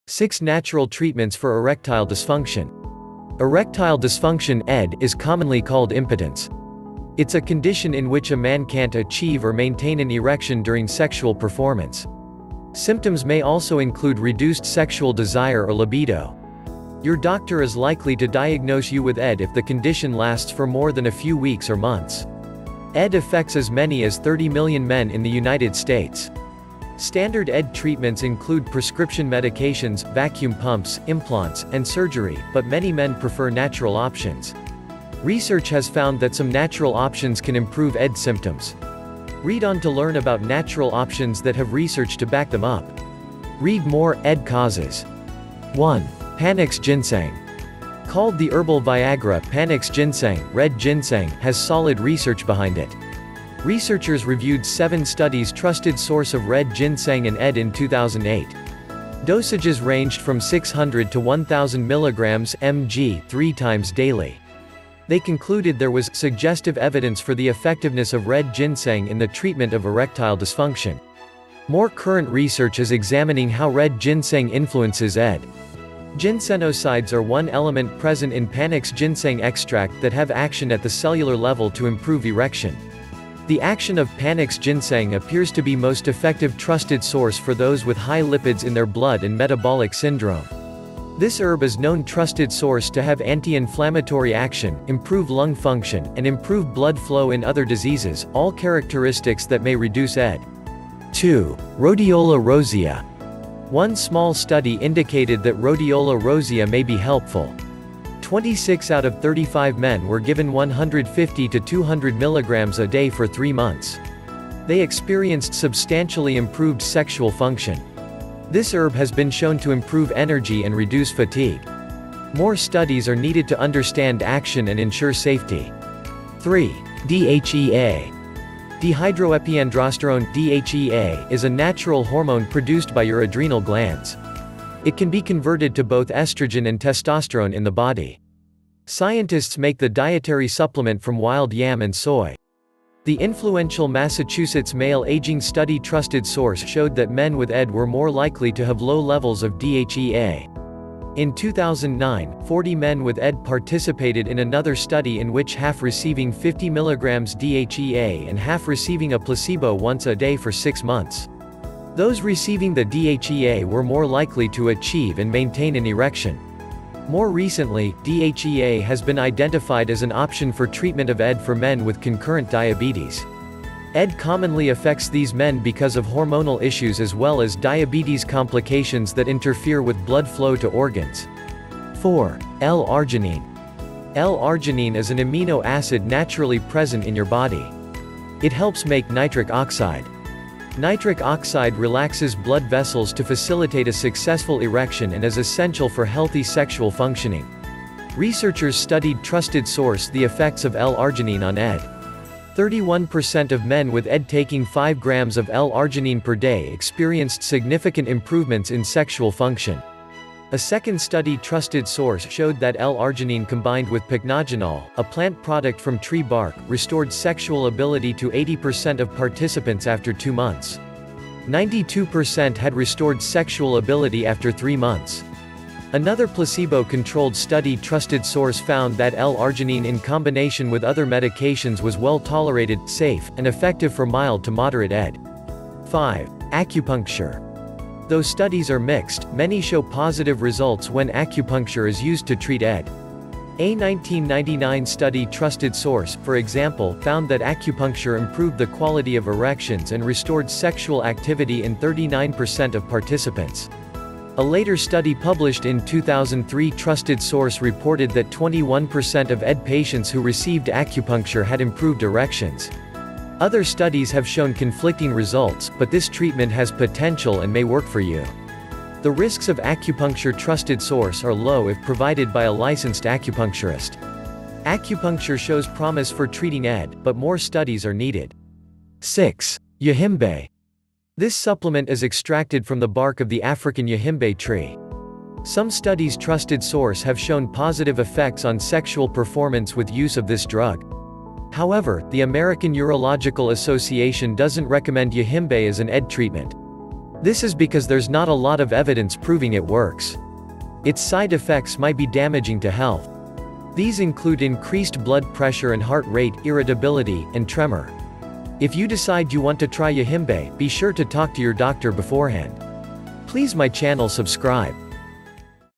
6-natural treatments for erectile dysfunction
6 Natural Treatments for Erectile Dysfunction

Panax ginseng.Rhodiola rosea.DHEA.L-arginine.Acupuncture.Yohimbe.Other natural treatments.Talk to your doctor.